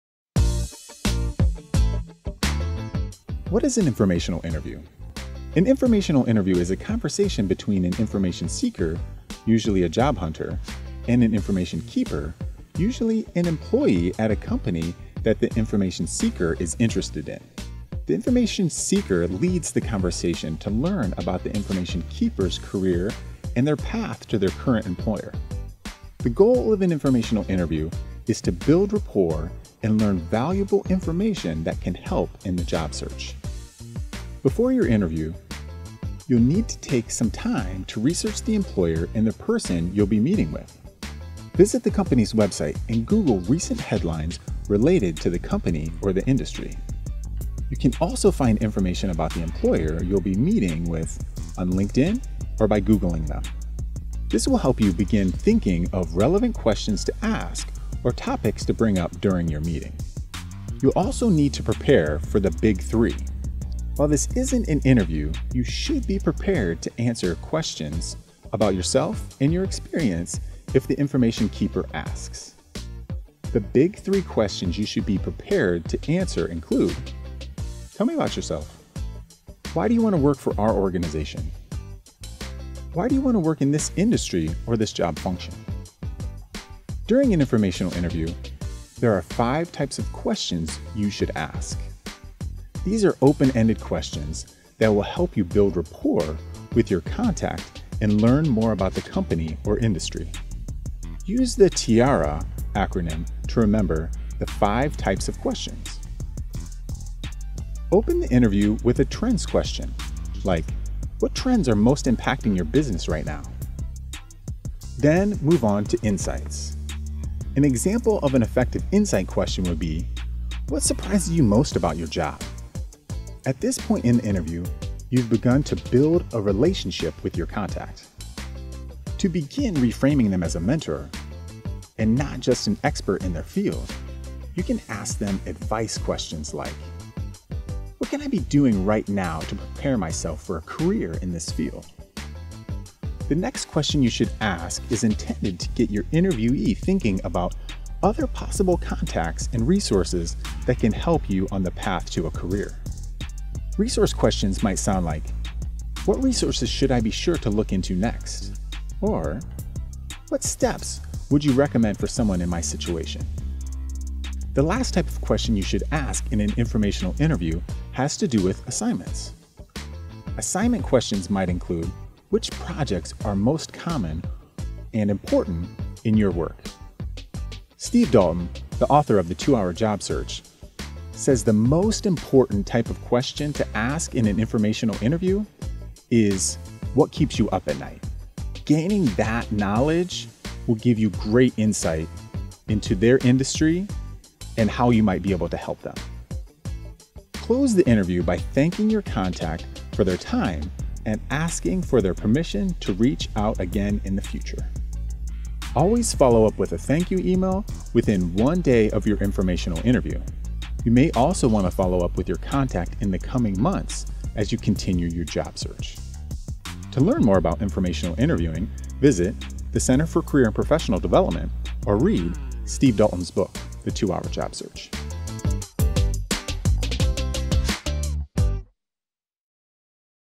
Informational Interviewing Crash Course
Learn how to conduct informational interviews and build professional relationships that can take your career to the next level.